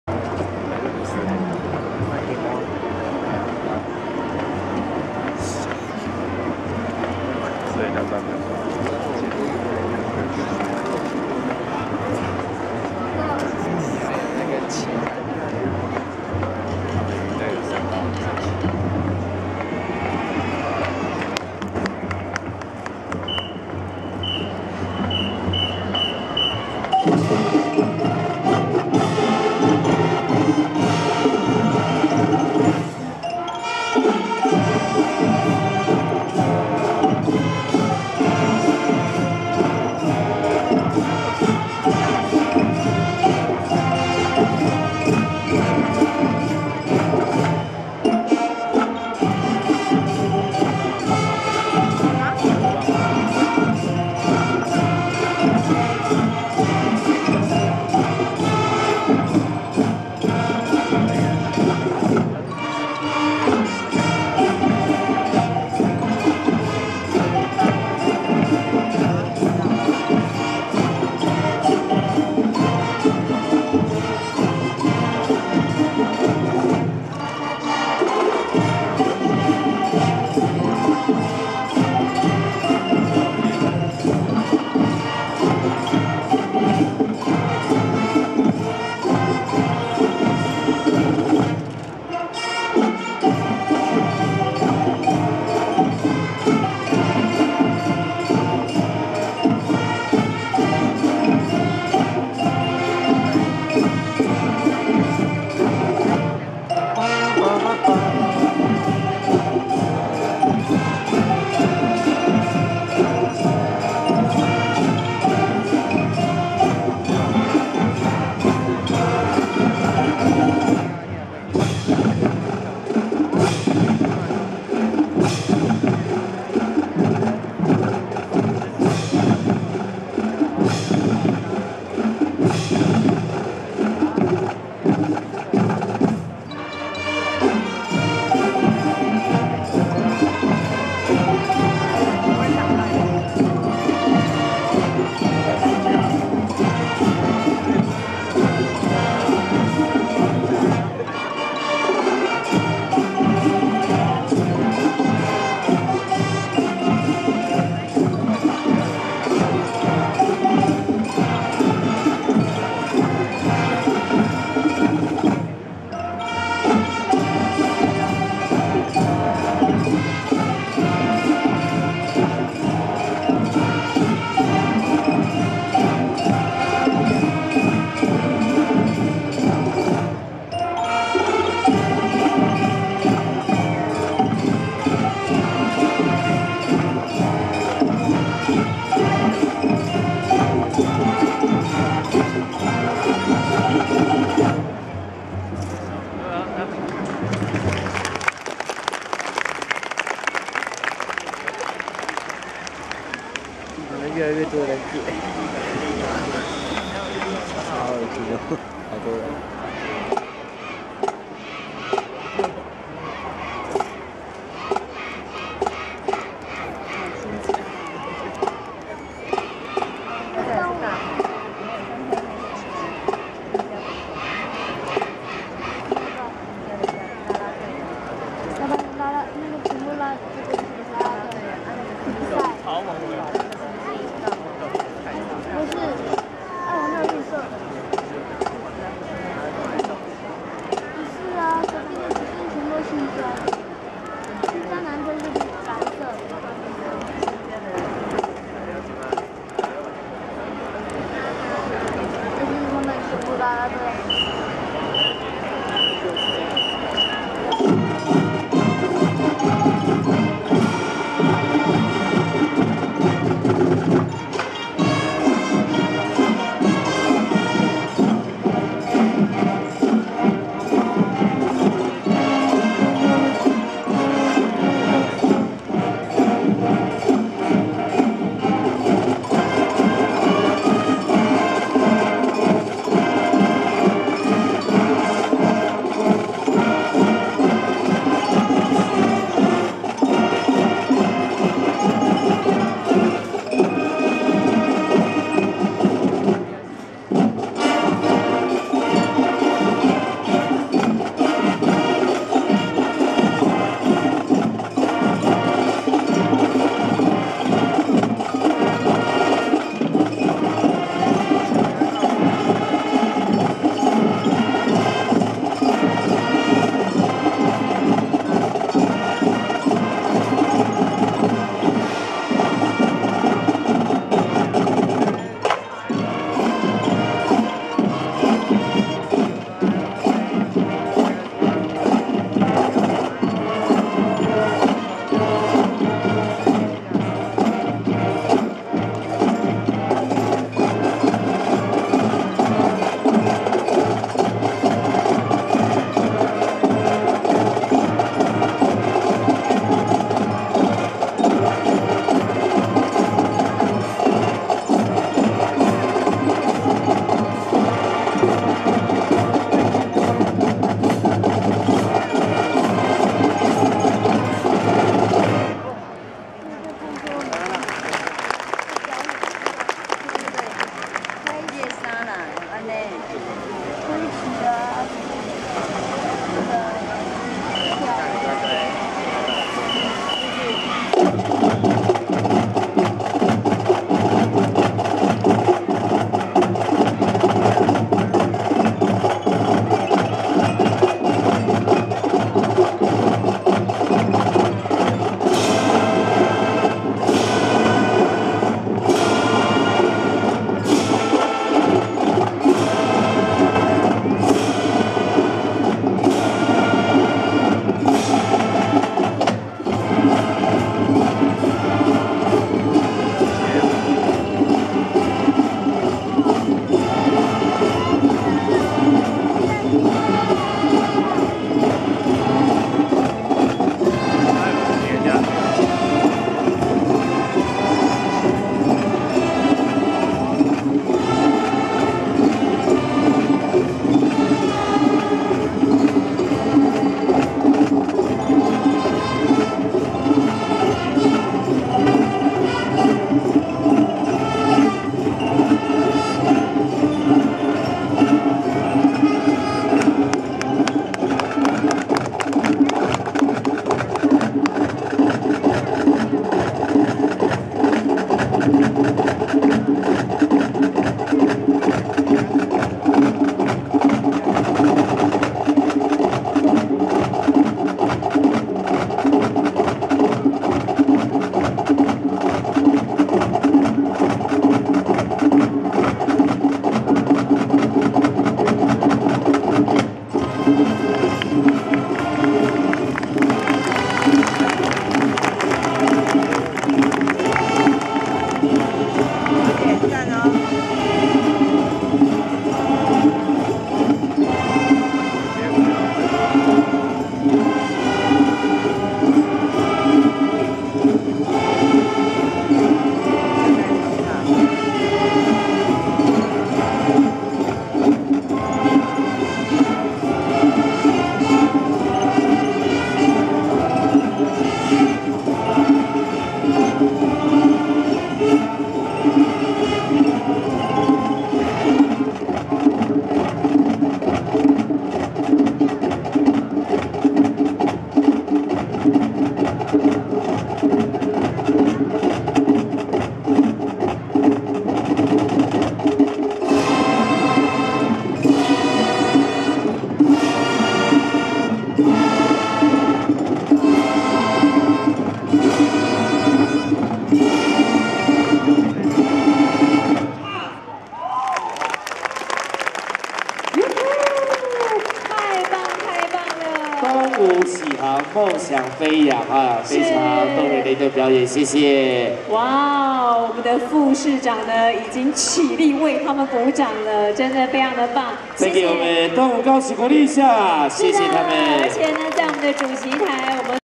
2010 嘉義市管樂節（走圖） - 嘉義東吳高職樂儀旗隊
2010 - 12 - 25　嘉義市國際管樂節　隊型變化（走圖） - 嘉義東吳高職樂儀旗隊